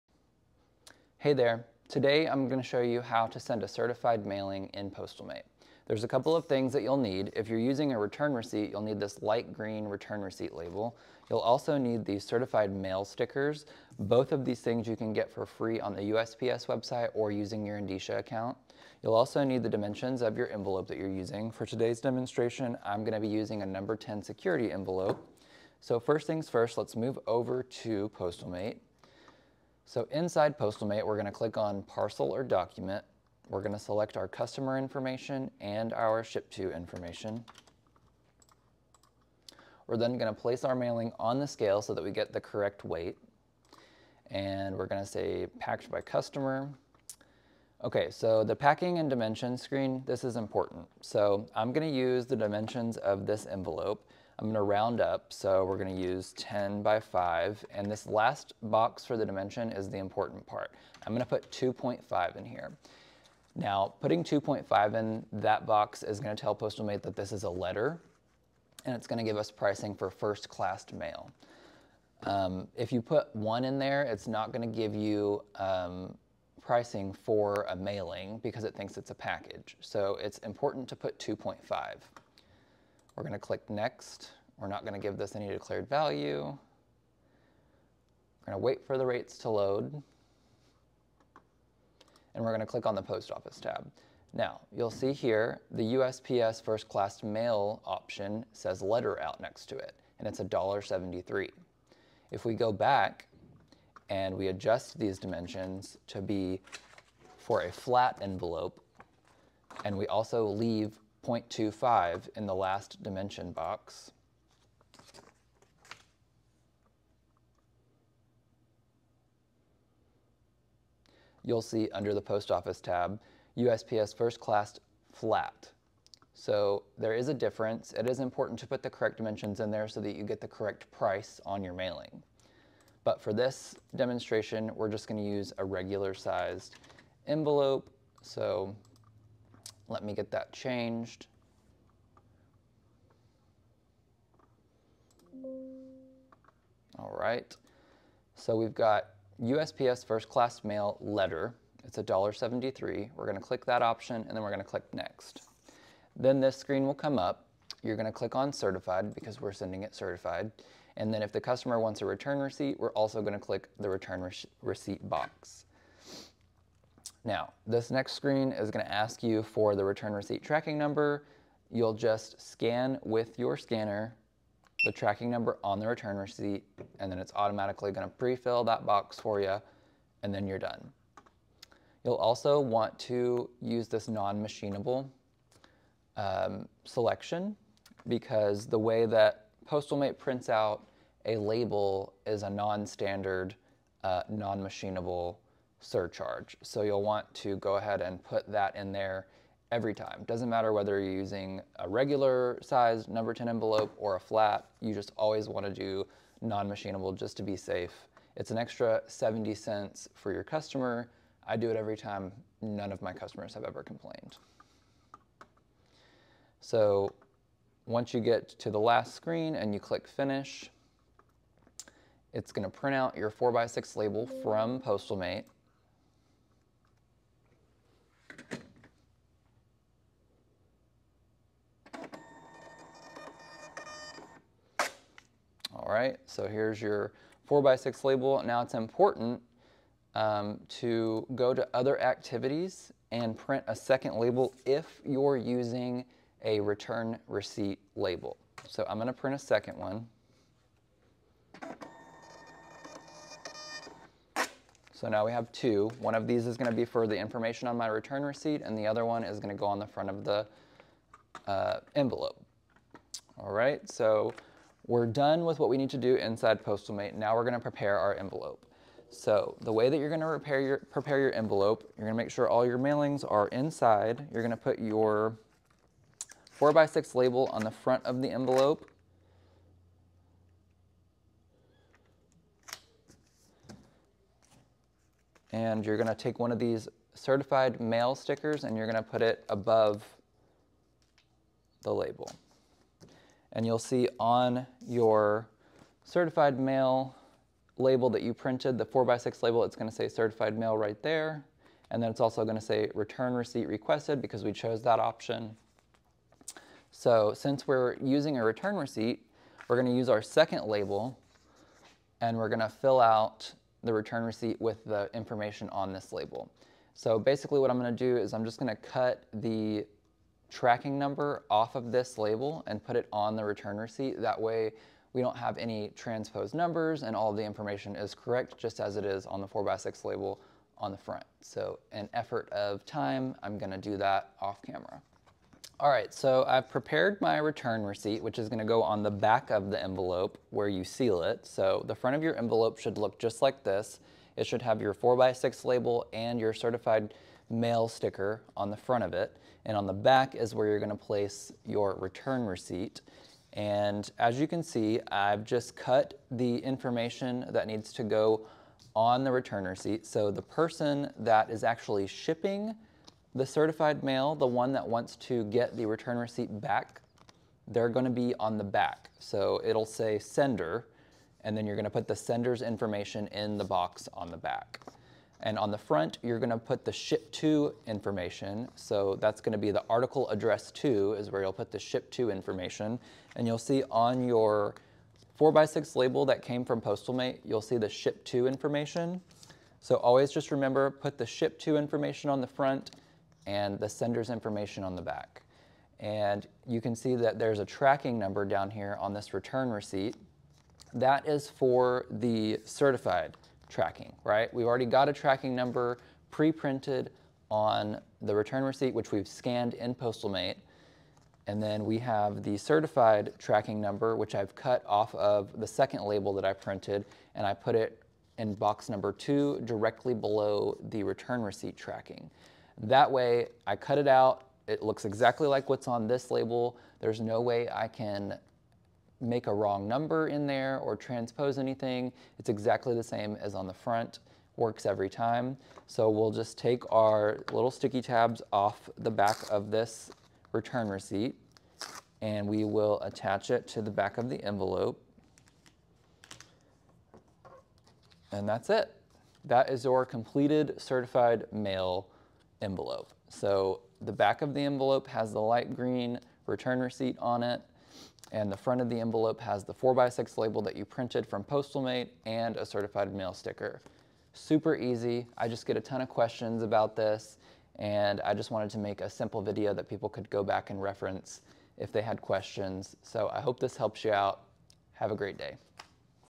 How to Process Certified Mail in PostalMate | Step-by-Step Tutorial
In this step-by-step tutorial, we’ll walk you through the entire process of creating, preparing, and sending certified mail using PostalMate software. Whether you’re a pack-and-ship store owner, employee, or just getting started with PostalMate, this video will help you understand exactly how to manage certified mail the right way.

✅ What you’ll learn in this video:

How to create a certified mailing in PostalMate

Step-by-step process for preparing certified mail

How to properly print labels and receipts

Best practices for handling USPS certified mail in your store

PostalMate is one of the most popular point-of-sale systems for pack-and-ship stores, and certified mail is a key service for many customers. With this tutorial, you’ll gain confidence in offering accurate, reliable certified mailing services every time.

📌 Perfect for:

New PostalMate users

Pack-and-ship store employees

Independent shipping store owners

Anyone looking for a refresher on certified mail